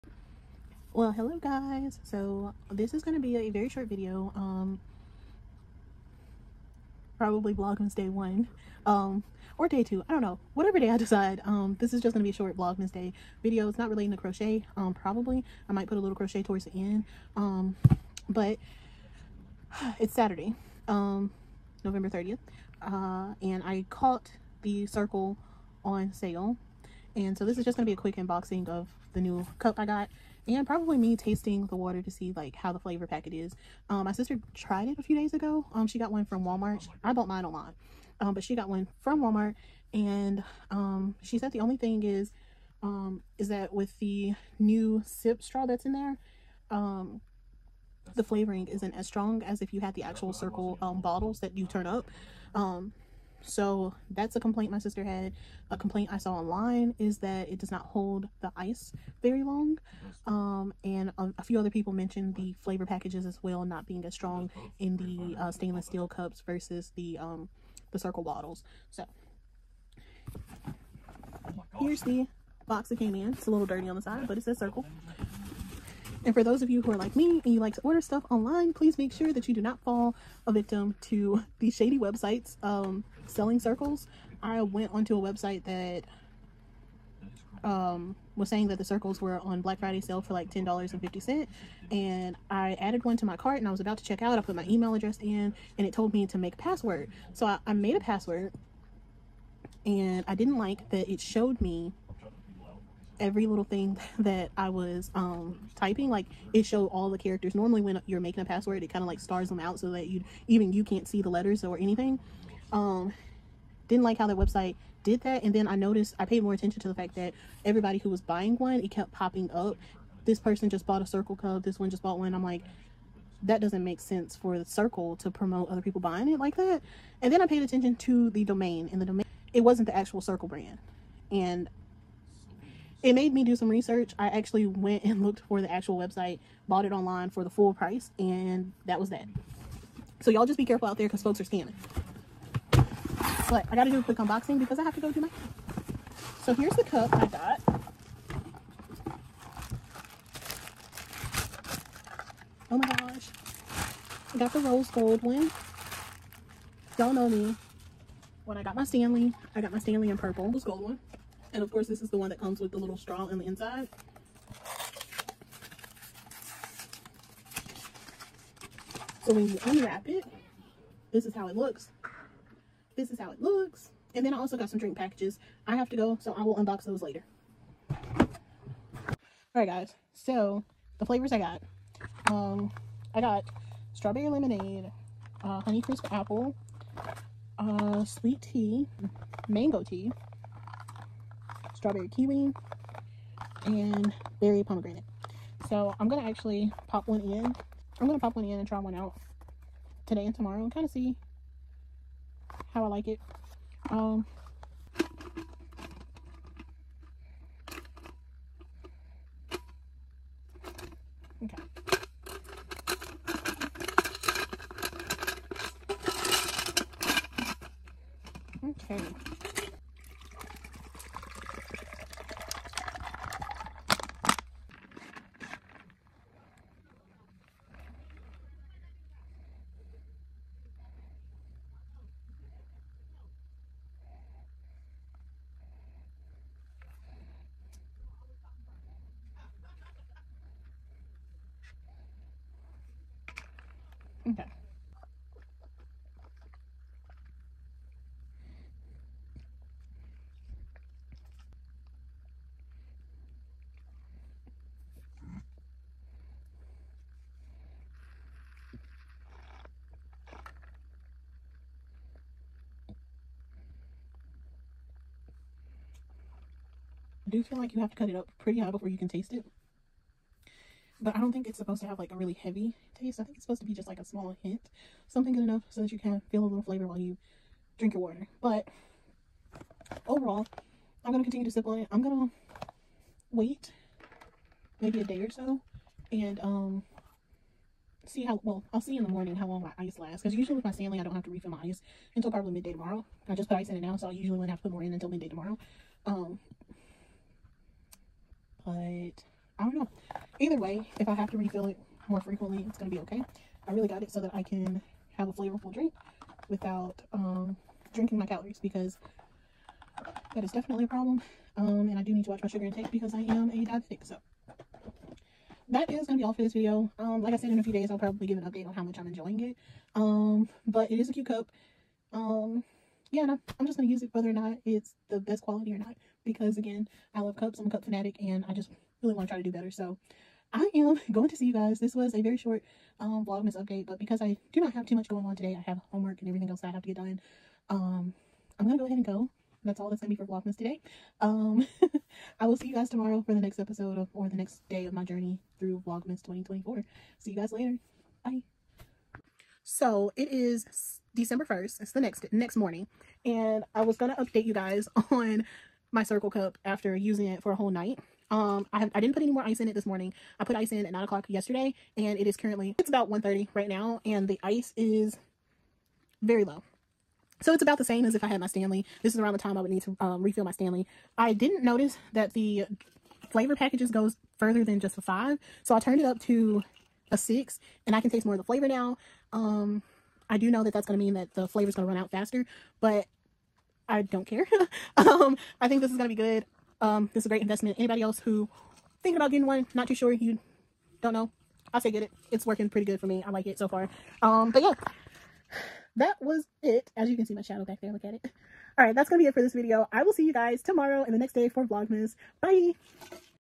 Unboxing my Cirkul StrawSip Tumbler| vlogmas Day 3- NOT crochet related
Hey guys! I finally caved in and bought a cirkul! Unfortunately, today’s vlogmas video is not crochet related but I do want my channel to be about my life and not just my crochet journey.  I hope you enjoy!

I will be uploading all through December!